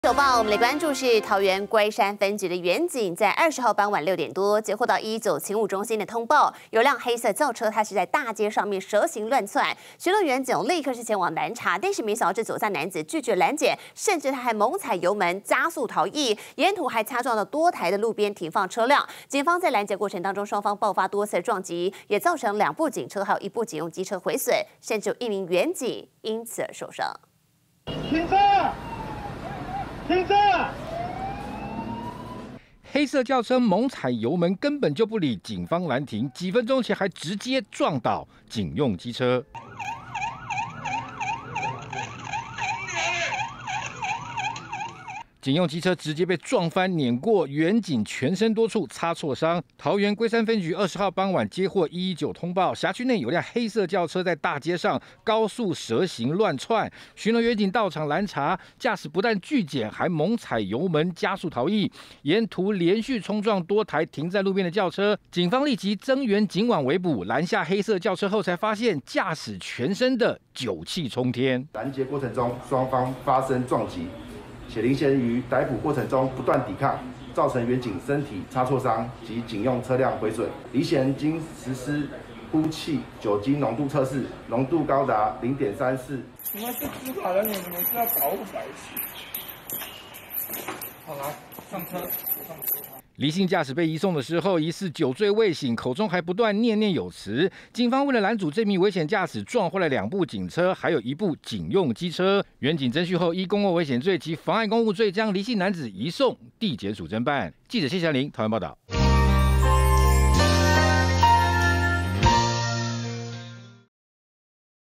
又是酒駕惹禍！ 男開車蛇行亂竄 拒檢還踩油門「撞警察」
桃園龜山分局員警20日傍晚接獲119勤務中心通報，有輛轎車在街上蛇行亂竄！巡邏員警前往攔查，不料駕駛不但拒檢，還猛踩油門撞倒騎機車的員警逃逸，沿途還擦撞多台路邊車輛。警方攔截過程中雙方發生多次撞擊，還有1名員警因此受傷，結果又是酒駕惹的禍！